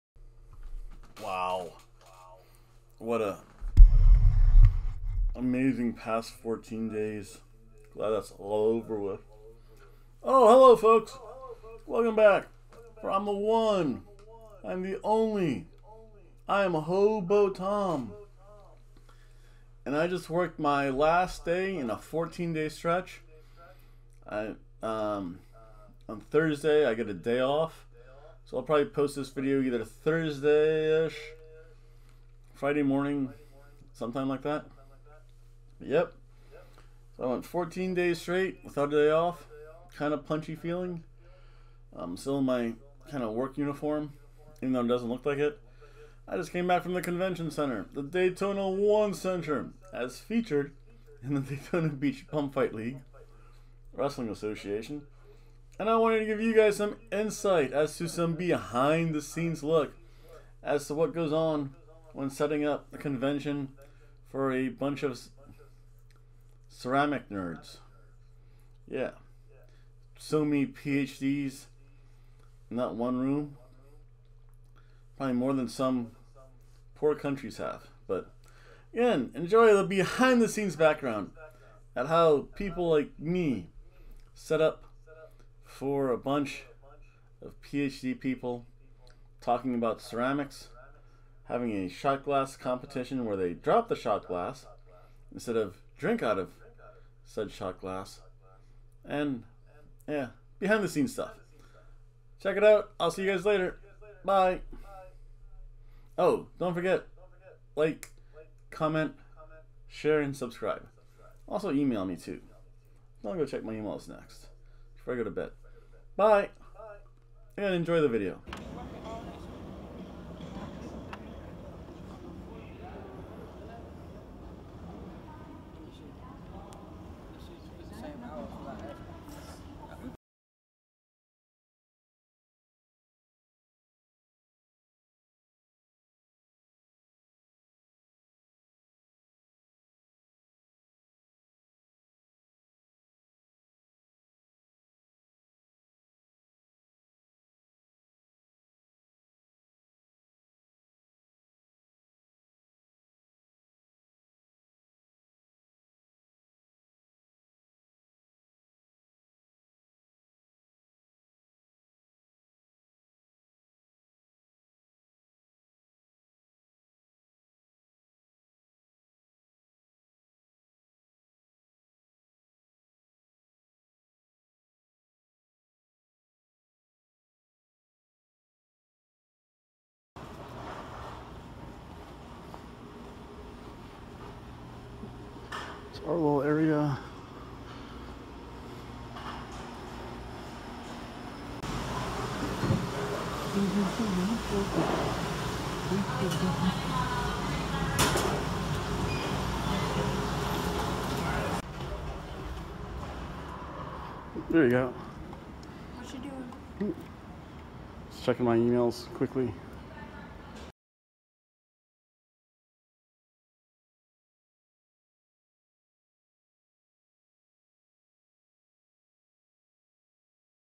Behind the NERDS at a Ceramic's Convention: Behind the black curtain at the Ocean Center
Another Behind the scenes, from the 1 and only HOBO TOM